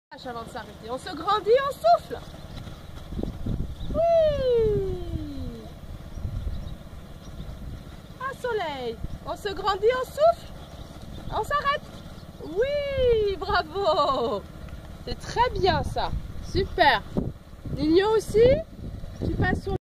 L'exercice de la bâche
Les petits cavaliers de l'école d'équitation font leur petite démo!